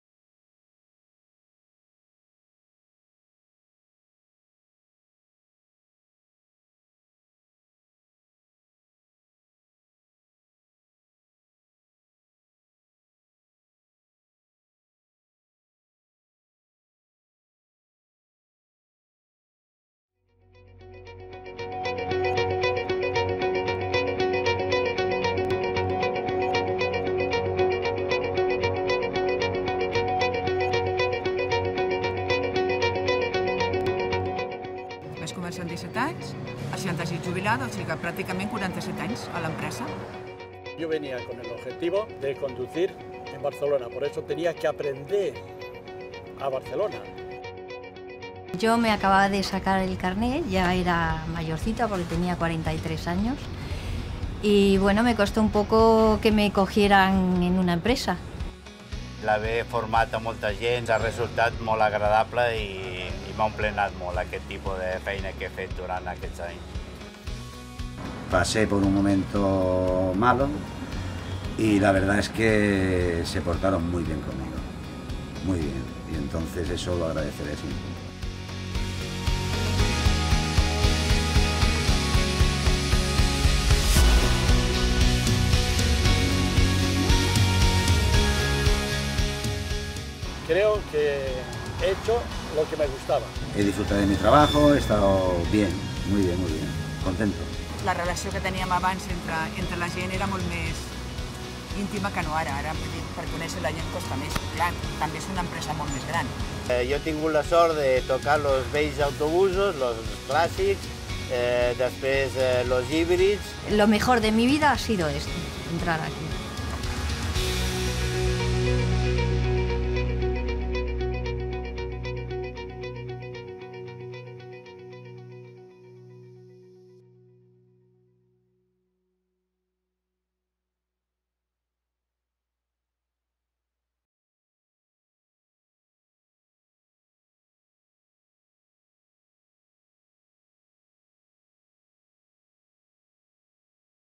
"Tu inspires TMB. L'experiència de la gent gran, el nostre valor present"
Per commemorar el Dia Internacional de la Gent Gran la companyia llança la campanya “Tu inspires TMB. L’experiència de la gent gran, el nostre valor present”, donant veu a 5 treballadors i treballadores de l’empresa de bus, en conversa amb tres persones grans que viuen a Barcelona.